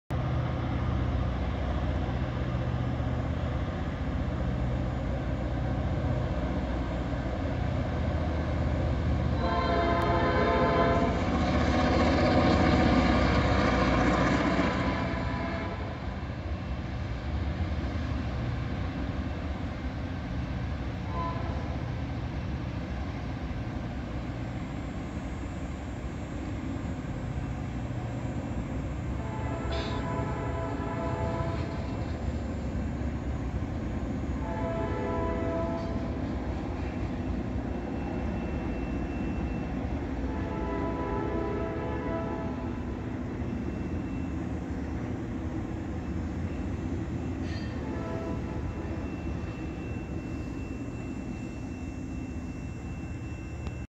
IC 9629 Leads CN Local East Through Brockville, Ontario
IC #9629 leads the local through Brockville, Ontario East to do it's day's switching. The trailing unit was a faded paint CN #4799. They had 23 mixed cars in tow. Caught mw by surprise, hence the wobbliness.